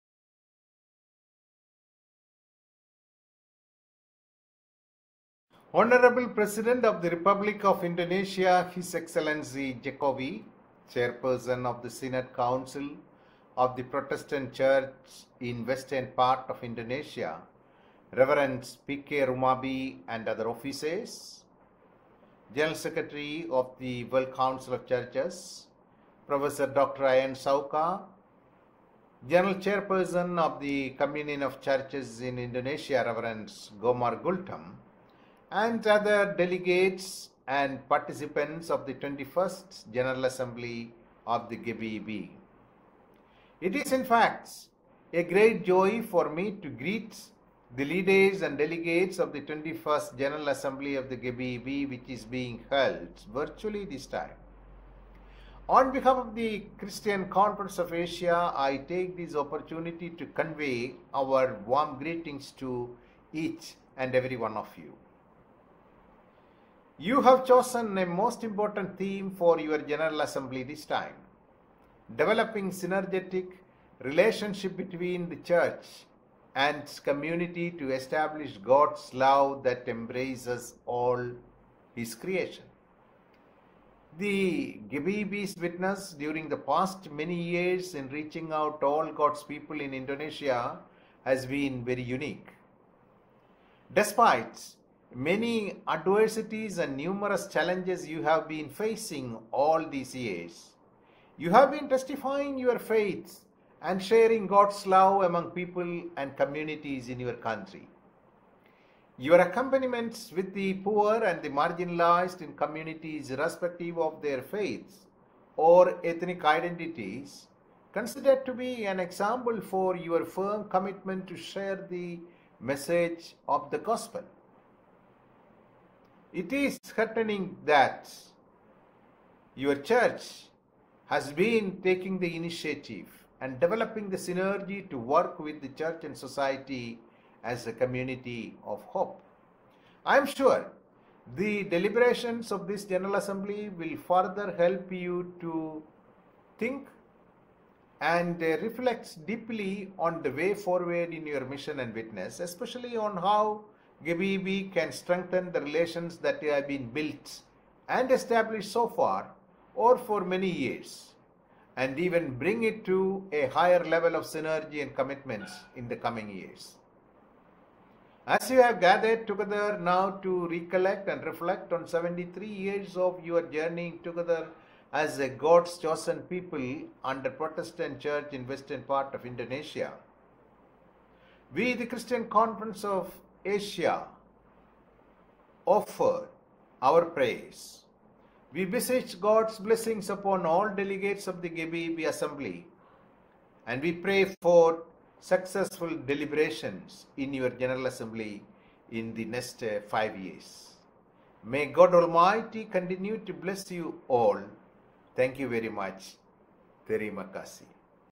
CCA General Secretary's message to 20th General Assembly of GPIB Synod Assembly
CCA General Secretary's message to 20th General Assembly of Gereja Protestan di Indonesia bagian BARAT (GPIB) Synod Assembly (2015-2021)